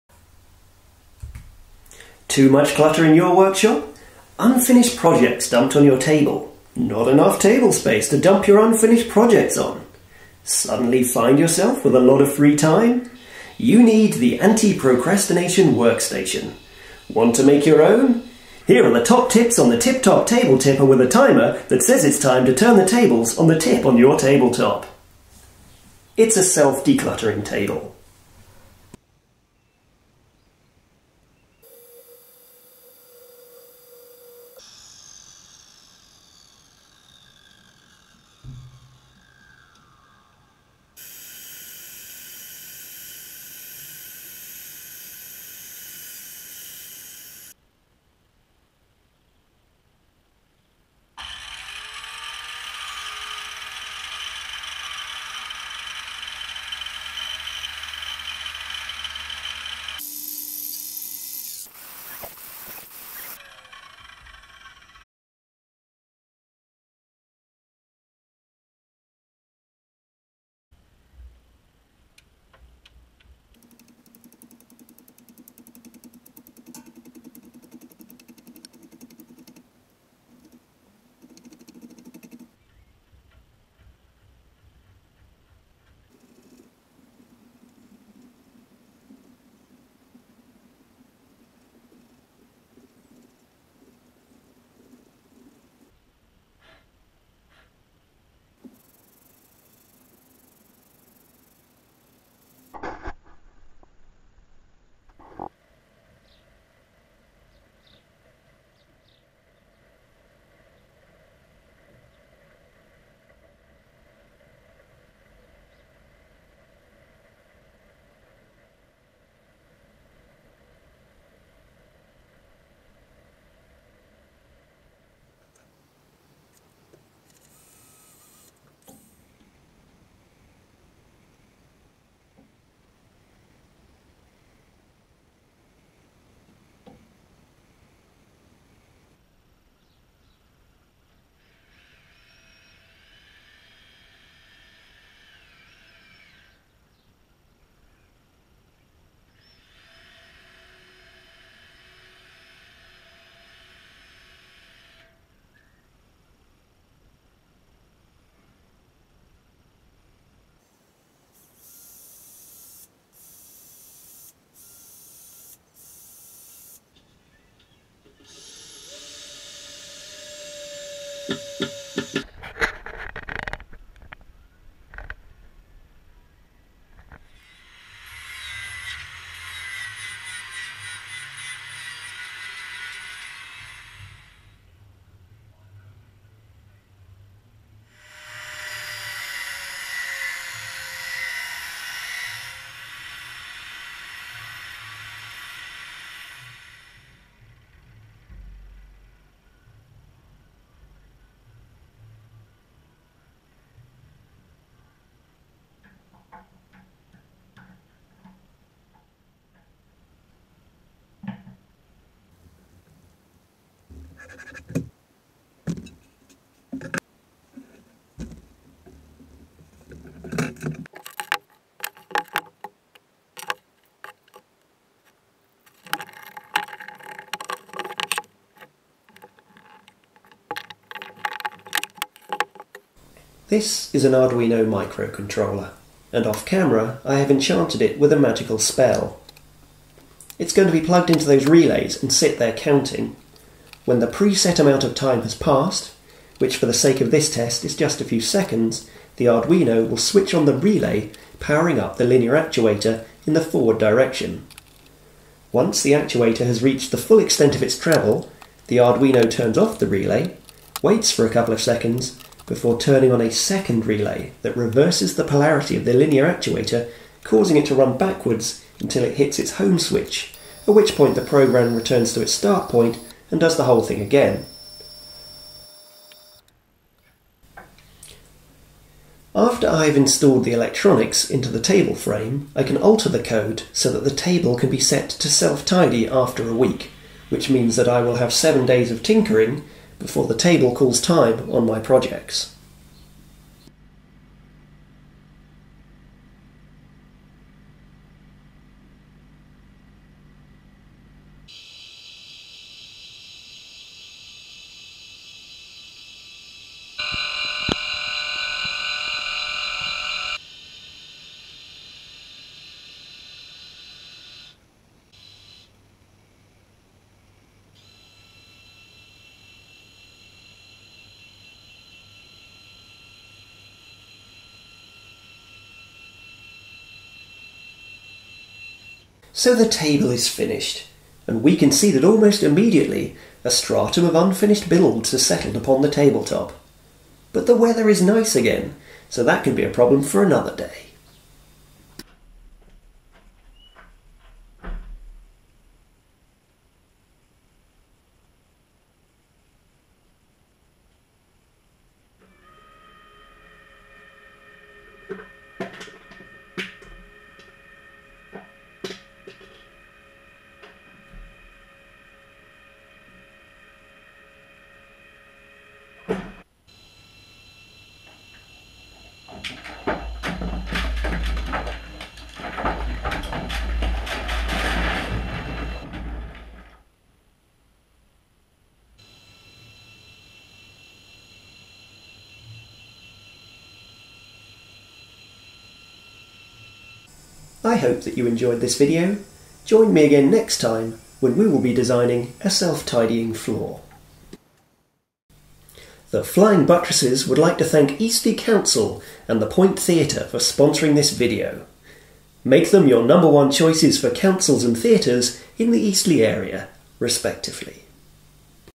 The Anti-Procrastination Work Station
Too many unfinished projects on your desk?  Too little desk space to finish any of those projects?  You need the Anti-Procrastination Workstation...
Commissioned during the lockdown of 2020 by Eastleigh Council and The Point Theatre Eastleigh